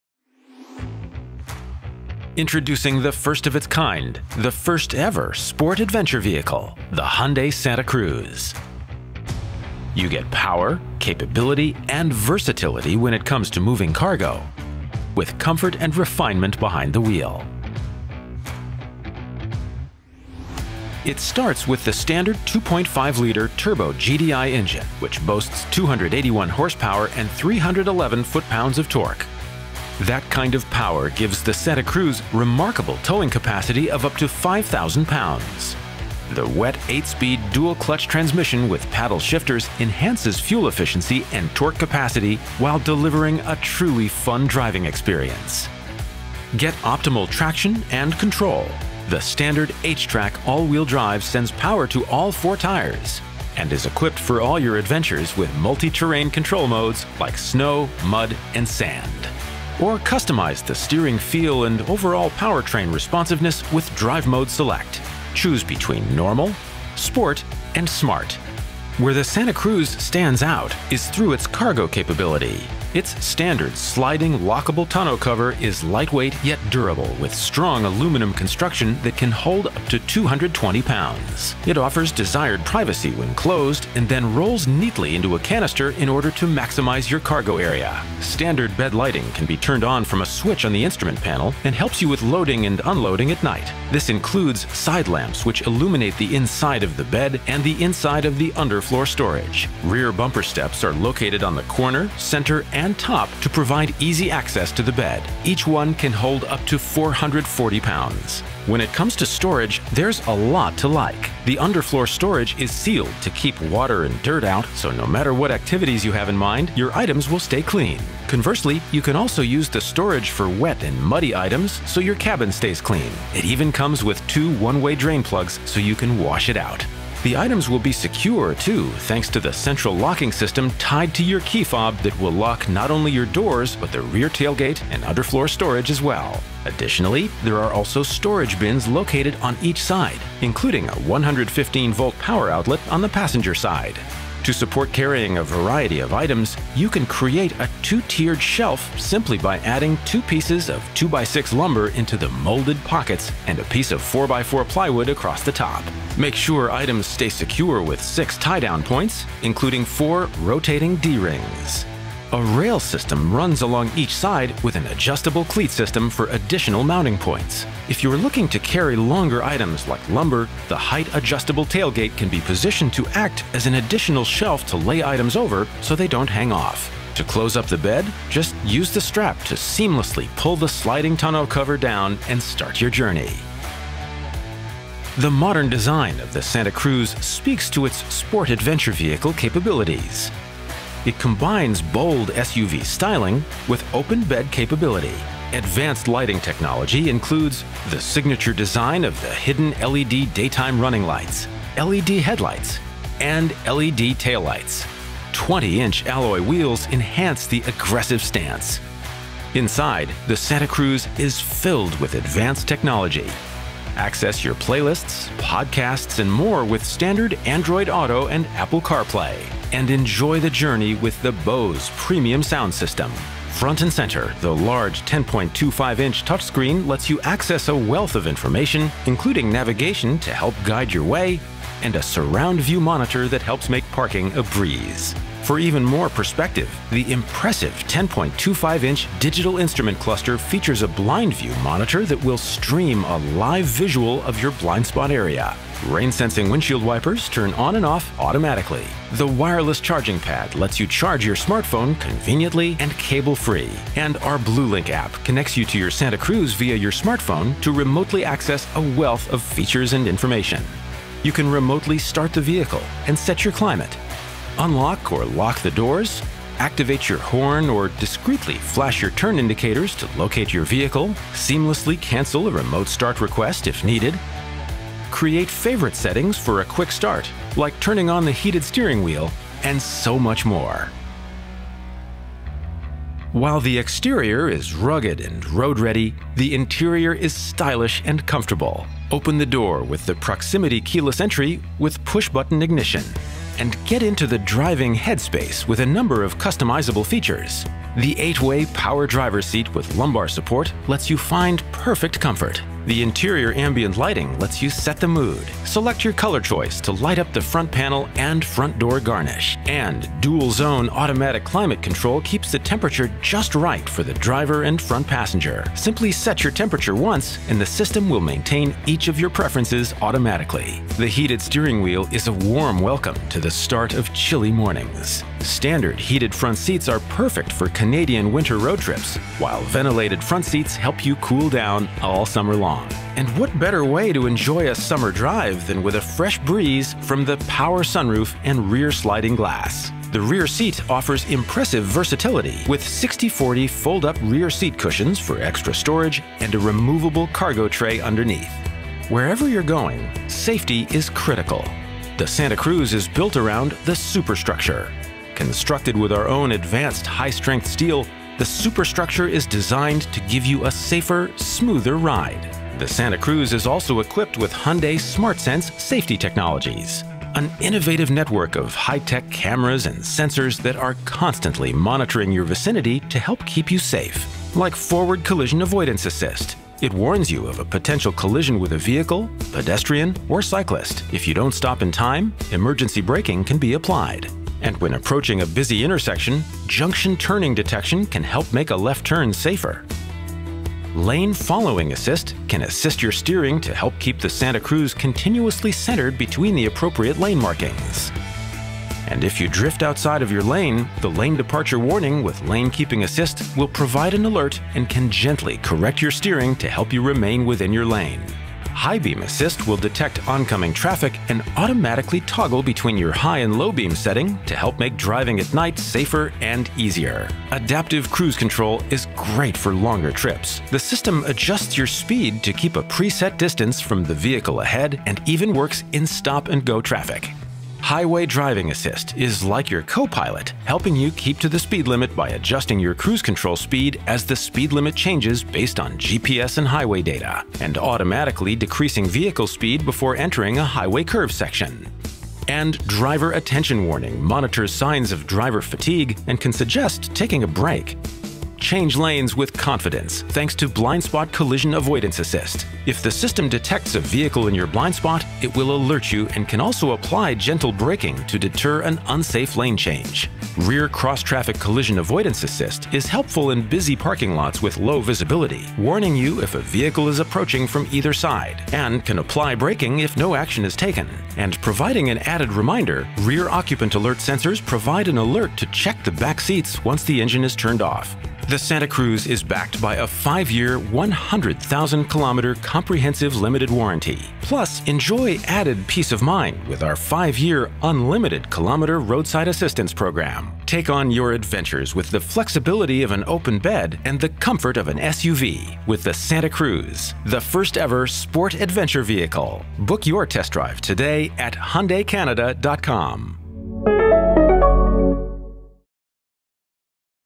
2022 SANTA CRUZ | Explore the product | Hyundai Canada
Discover the SANTA CRUZ, the first-ever Sport Adventure Vehicle that combines SUV style with open-bed versatility. Explore the features and enjoy power, capability and flexibility with comfort and refinement behind the wheel.